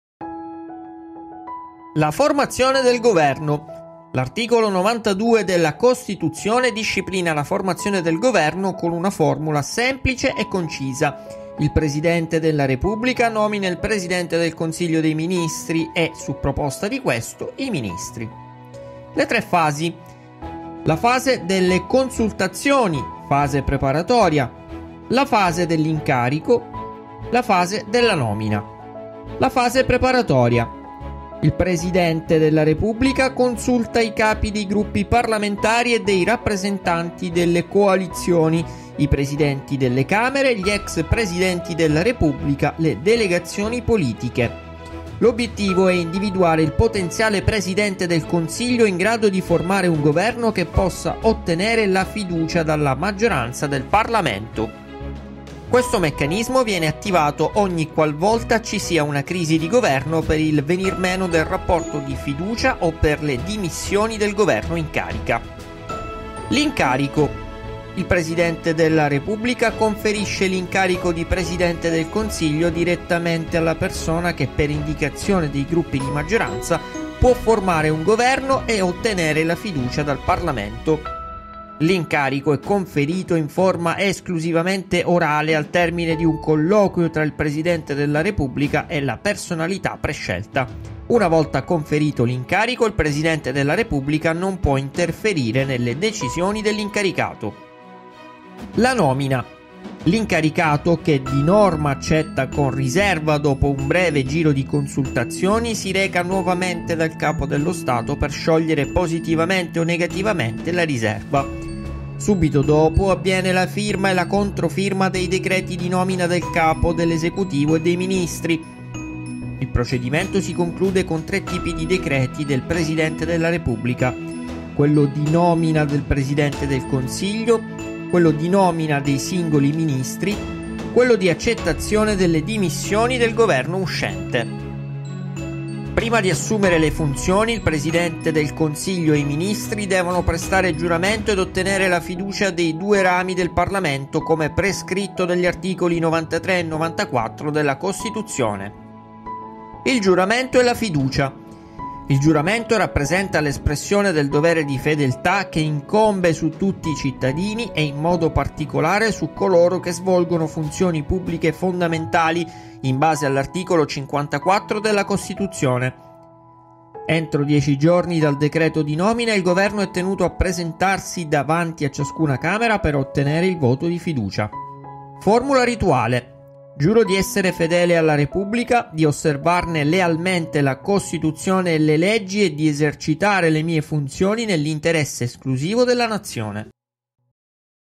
La formazione del Governo: tutte le fasi passo dopo passo
governo educazionecivica tecnicadellascuola Come si forma il Governo italiano? Chi sceglie il Presidente del Consiglio dei ministri? Perché non sempre la formazione di un nuovo esecutivo passa dalle elezioni?